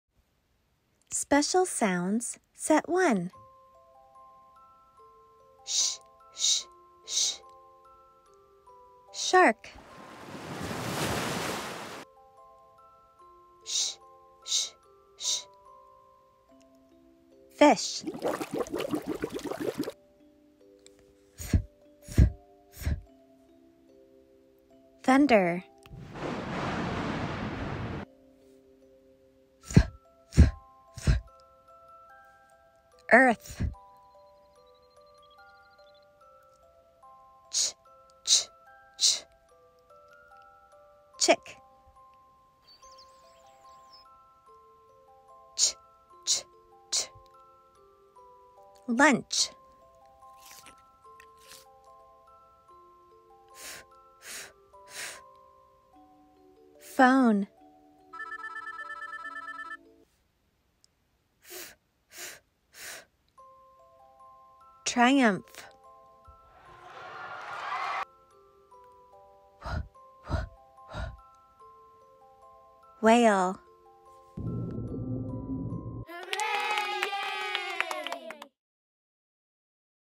Consonant Blends | ABC Learn With Me
Blending two consonants together to make a new sound.